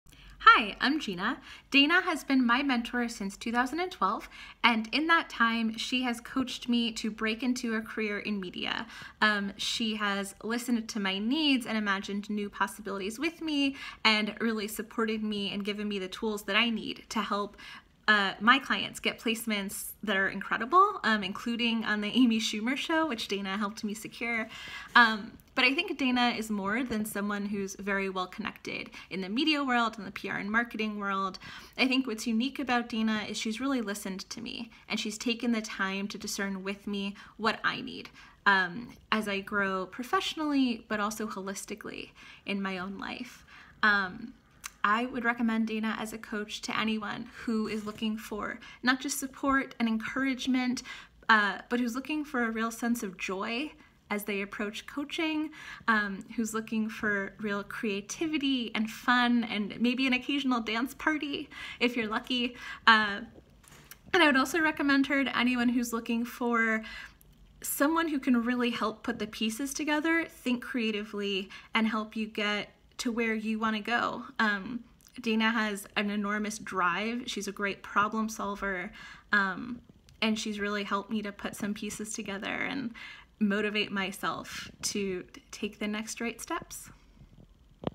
Glowing Testimonial from Gina Ciliberto
A huge thank you and shout out to our dear friend and mentee Gina Ciliberto! It has been such a blessing to watch Gina grow and blossom into the brave writer, collaborative creator and motivated entrepreneur, she is today. We celebrate Gina and her lifelong journey in becoming her most empowered and authentic self. Go Gina!